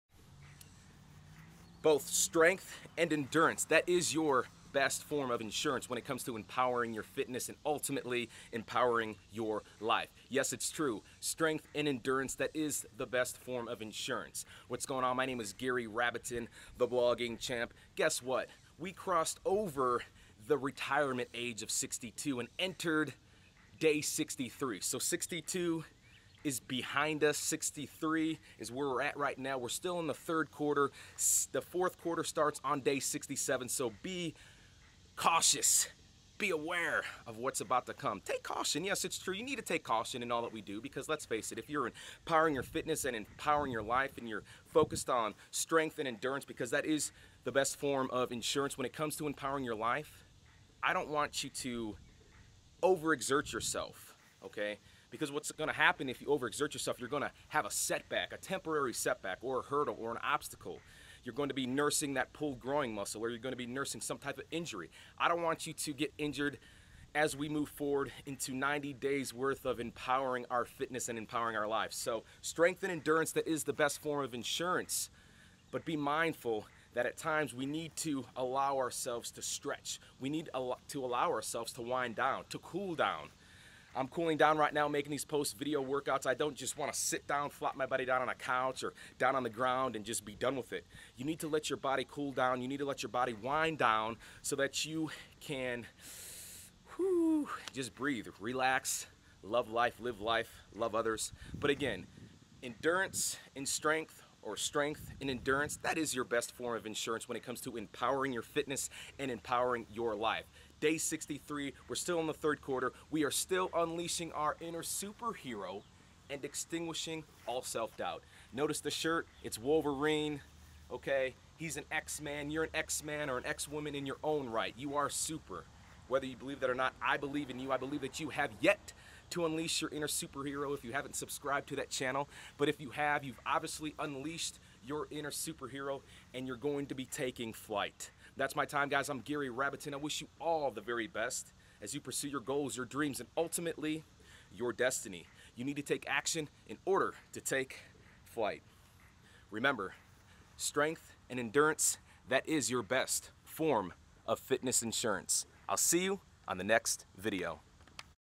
Empower Fitness Network Day 63 Post Workout Message By Gary Rabatin ( The Blogging Champ )
We just went past the retirement age.